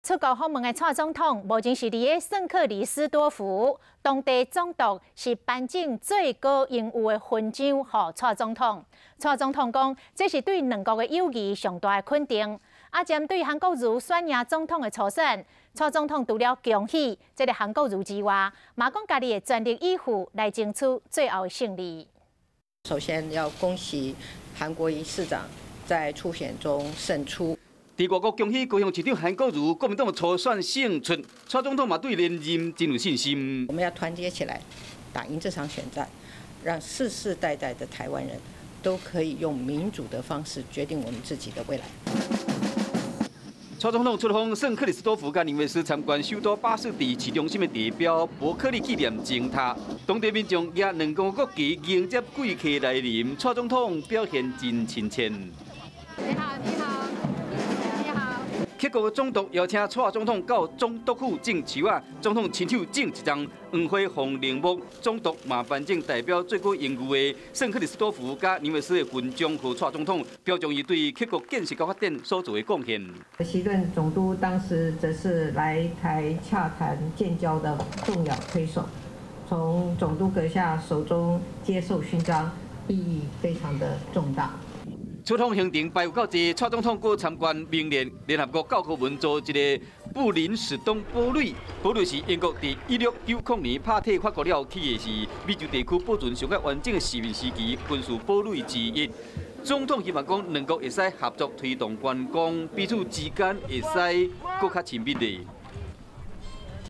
【台語新聞】兩國最大友誼肯定 總統獲頒克國勳章 | 華視新聞 20190716
總統蔡英文目前正在聖克里斯多福及尼維斯訪問，獲得克國總督席頓，頒贈最高榮譽勳章，蔡總統表示這是對兩國長期友誼，最大的肯定！對於高雄市長韓國瑜，在國民黨總統初選中獲勝，她除了恭喜韓國瑜，也說自己會全力以赴，爭取最後勝利！